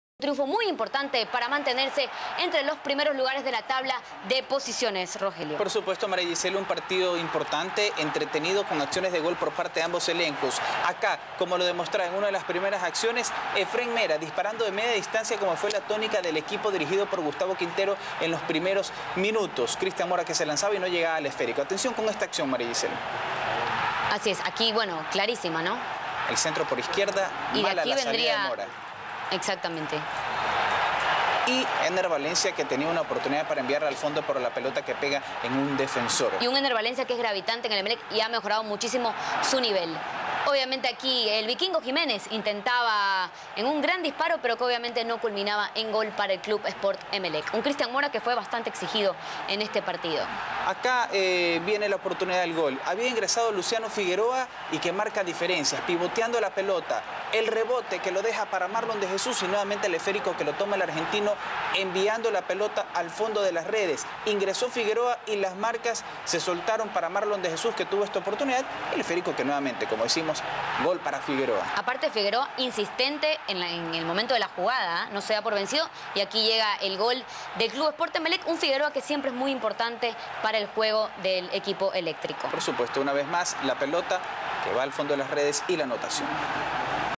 Figueroa le dio el triunfo a Emelec en Ambato
Los eléctricos nuevamente cosecharon un buen resultado en el Bellavista. El sábado derrotaron 0-1 a Macará.